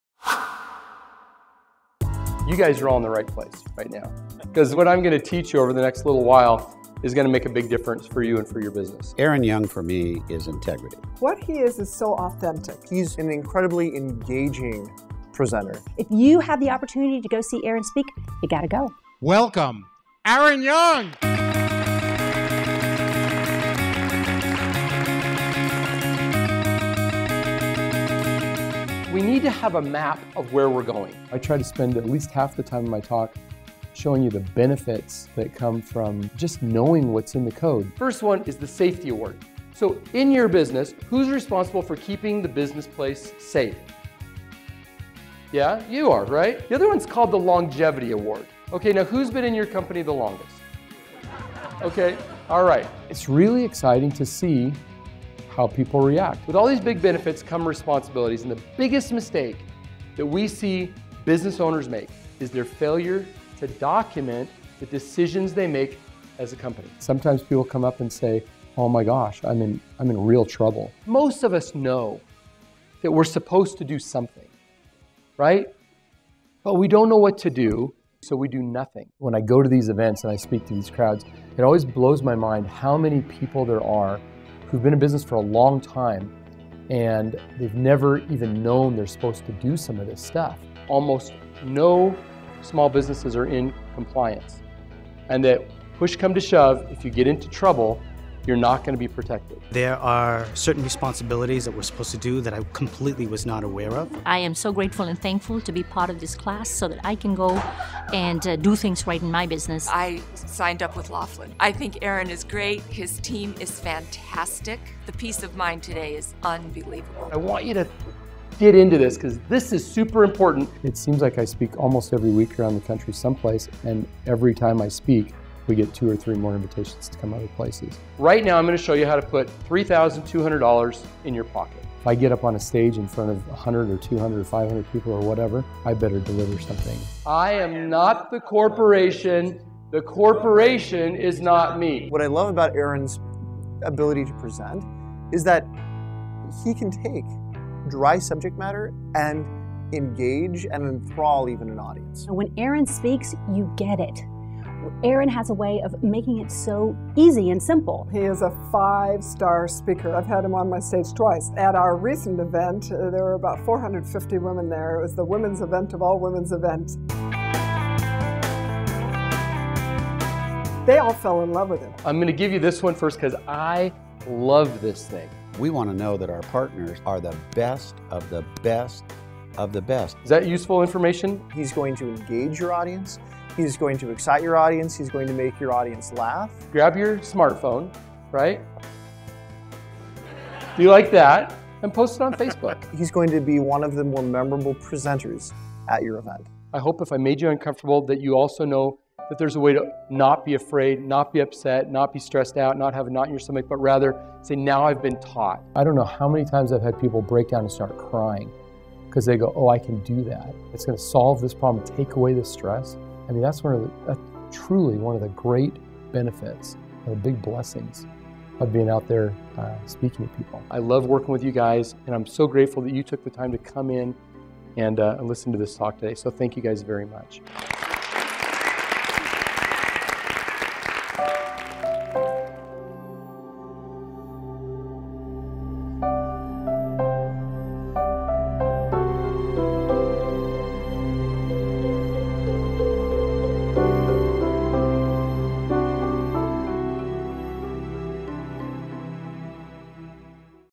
Book Aaron to speak at your next event.
Aaron Young teaches business owners to protect what they've built, spend pre-tax dollars and make their business an asset not just a glorified job.

As CEO of Laughlin Associates, Aaron has served as a Chief Strategy Officer for more than 80,000 business owners across a wide range of industries—from the largest yacht broker in the US, to franchisees, to electrical contractors, to medical professionals. Having witnessed both their common mistakes and common successes, he knows what works and what does not.

Drawing on his own entrepreneurial and corporate experience, as well as what he has observed with his diverse clientele, Aaron has developed a cutting-edge system to help small business owners level the playing field. He teaches them the six critical strategies that make the difference between failure and long-term success.

Learn more about booking Aaron.